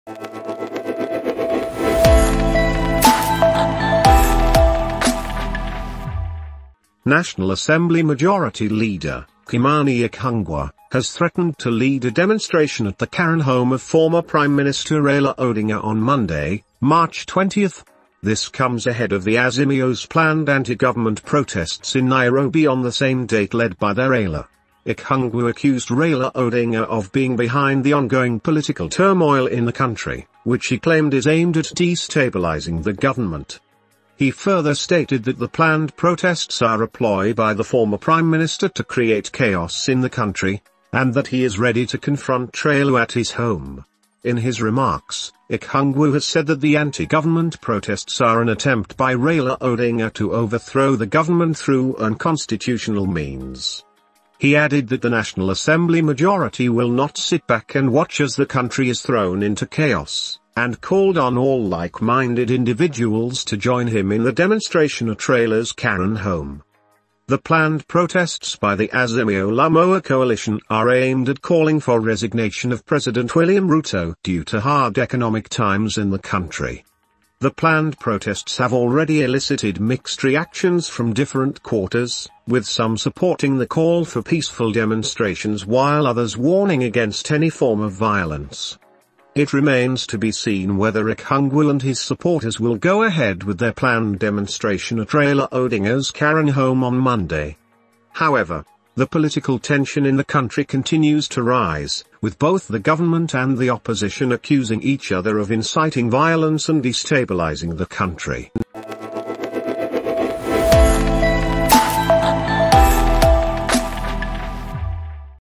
Demonstration In Raila Karen Home| News54
Majority Leader Kimani Ichung'wah Threatens to Demonstrate at Raila's Karen Home.

The Official news54 Channel ,Catch Up With Latest And Up-to Date News Making headlines Across Africa, Kenyanews.